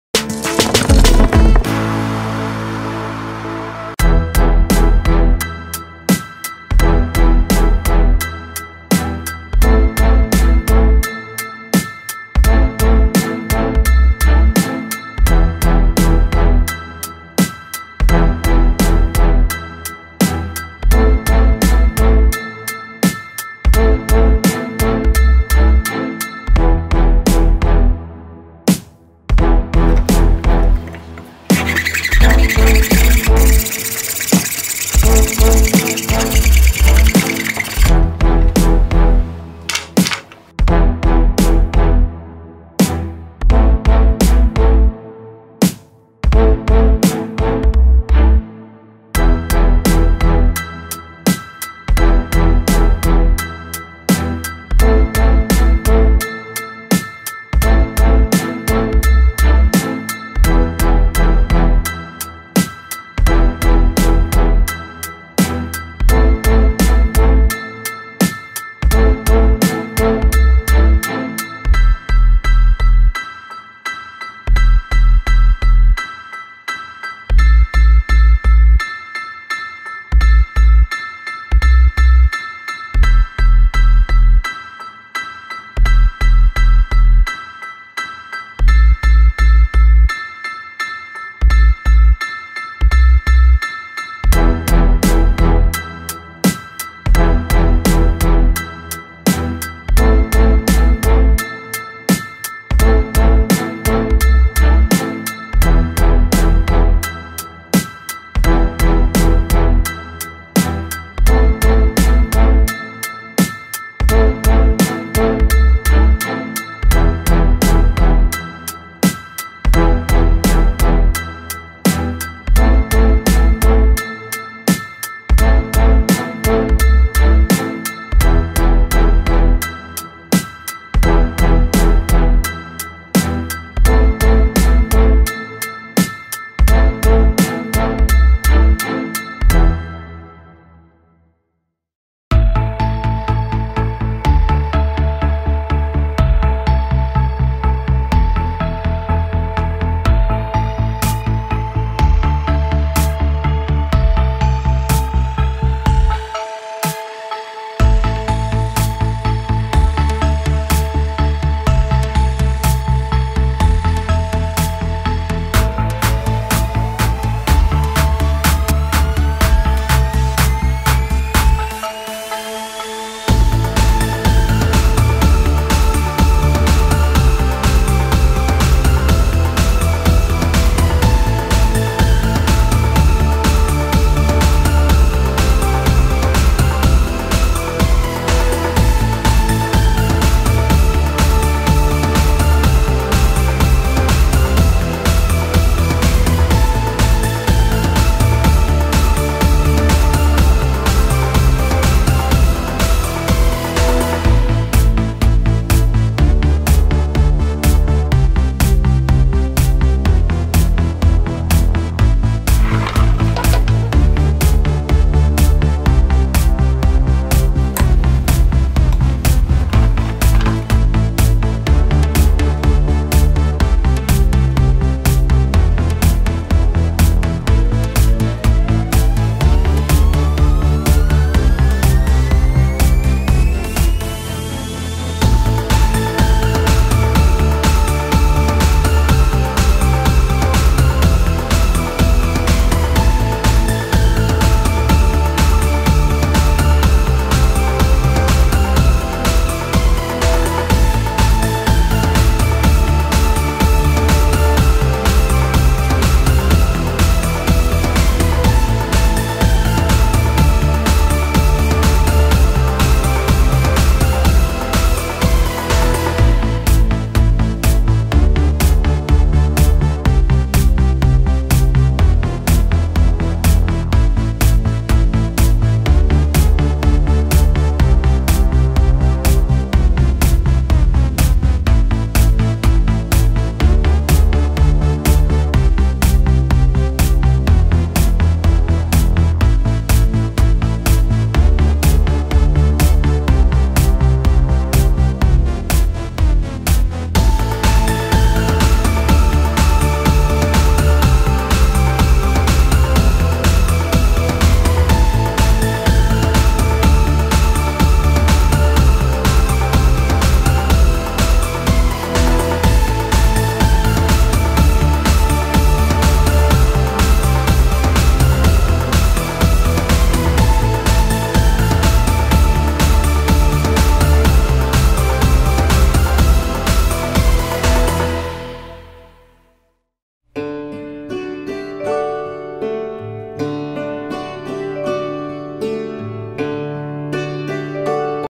Kartondan sevimli bir ev nasıl yapılır rüya evi projesi
Merhaba adam! Bu videoda size Karton Kullanarak İnanılmaz Malikane Yapmanın Adım Adım Adım Derleme ile Nasıl Yapılacağını göstereceğim.